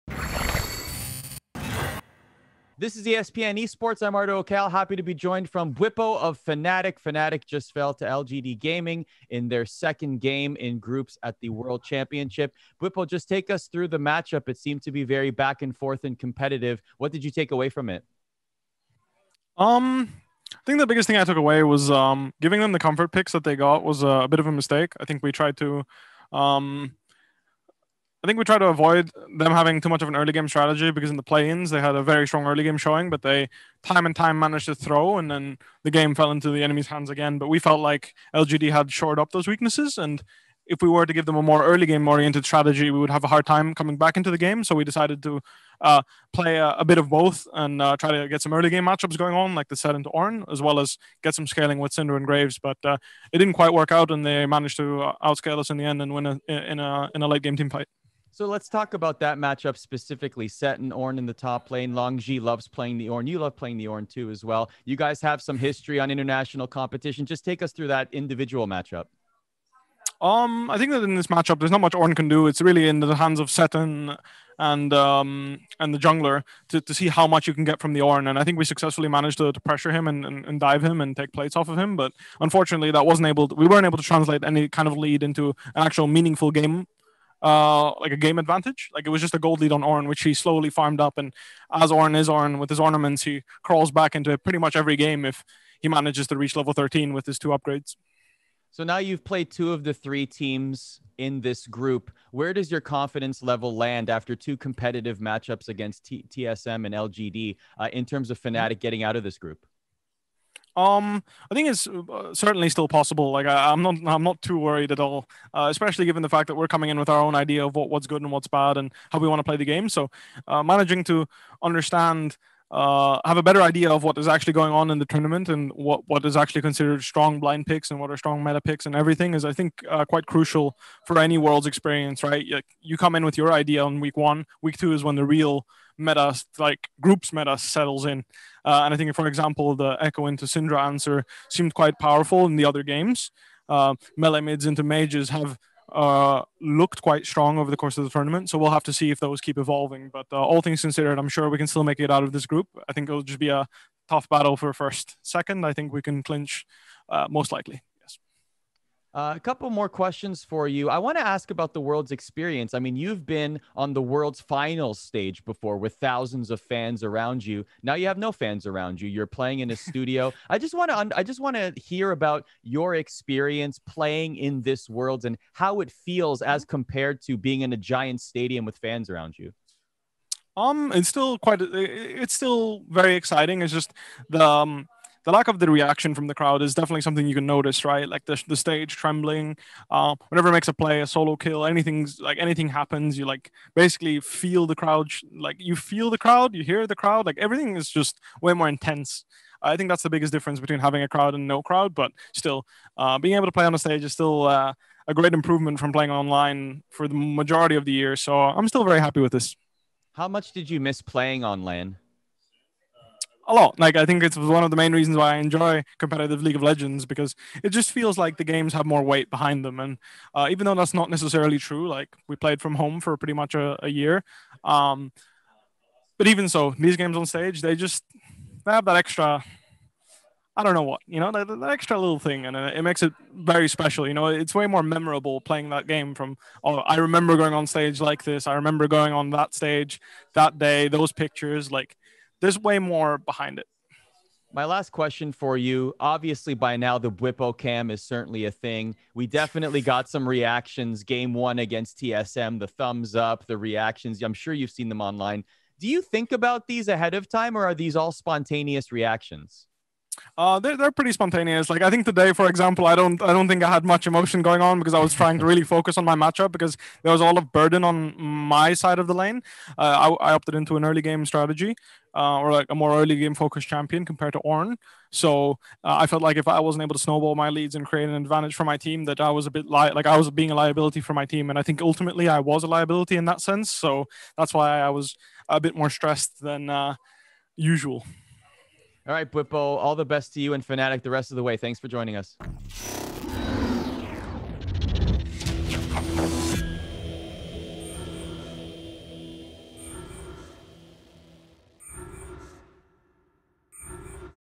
Bwipo talks Ornn and the Jungle meta at Worlds 2020 | ESPN Esports
Bwipo joins Arda Ocal to discuss Fnatic's performance at Worlds 2020 groups stage so far and his goals for the event moving forward.